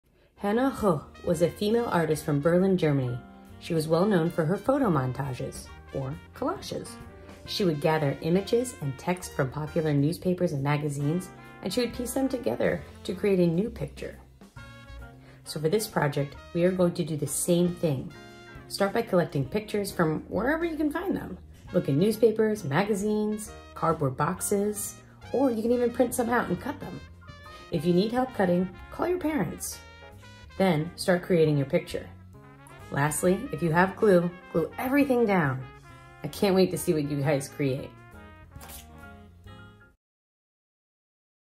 Hannah Höch
Short, fun kids art lesson.